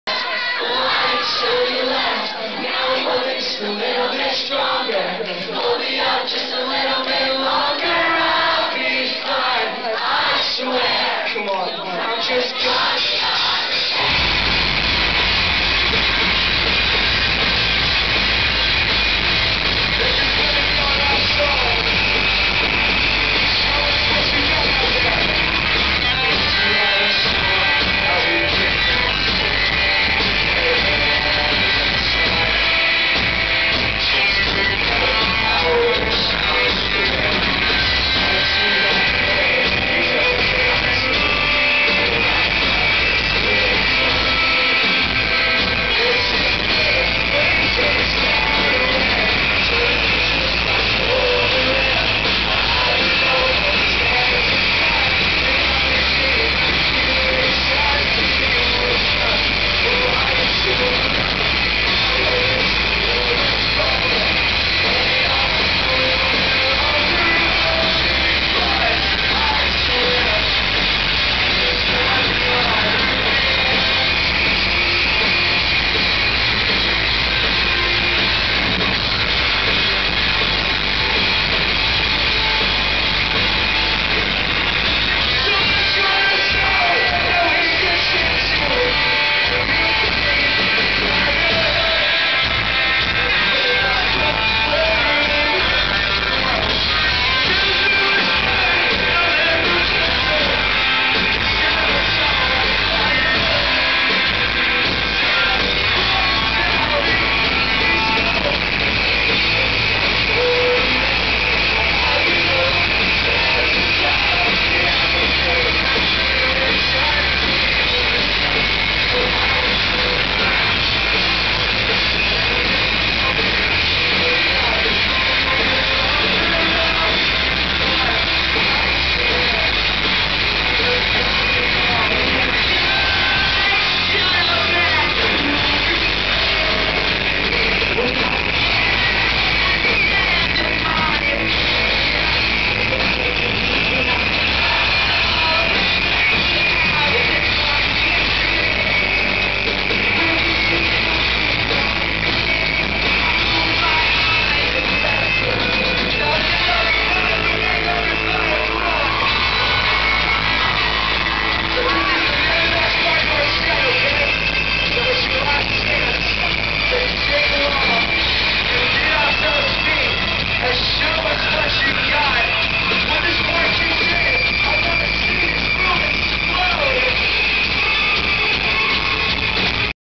Mayday Parade @ Workplay-Jersey
mayday's headlining tour 12-16-09 in birmingham with vega under fire, the dangerous summer, and there for tomorrow